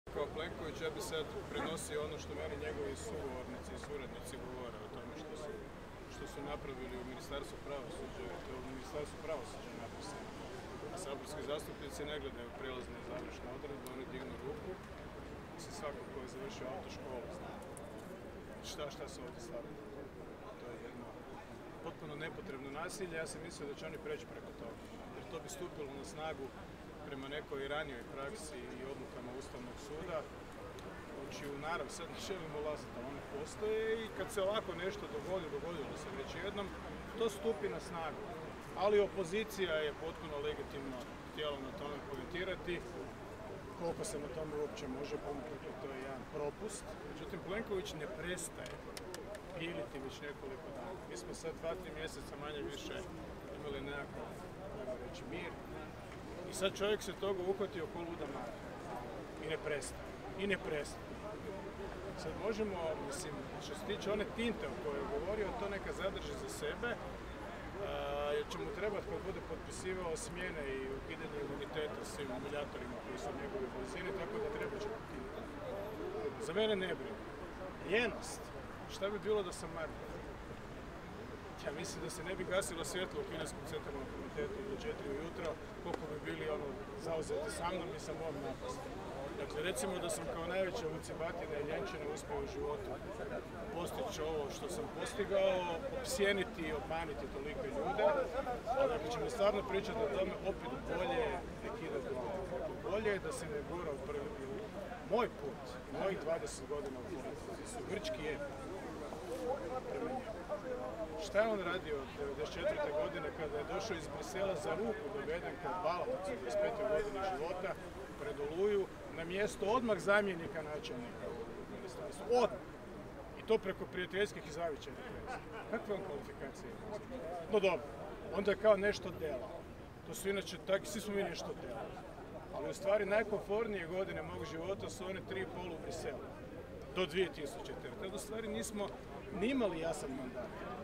Milanović: "Plenković ne prestaje piliti, tintu nek zadrži jer će mu trebati za potpisivanje smjena"
Predsjednik Republike i vrhovni zapovjednik Oružanih snaga Republike Hrvatske Zoran Milanović sudjelovao je na 32. obljetnici mobilizacije 144. brigade HV-a Sesvete kod spomen obilježja „Majka i dijete“ u Sesvetama.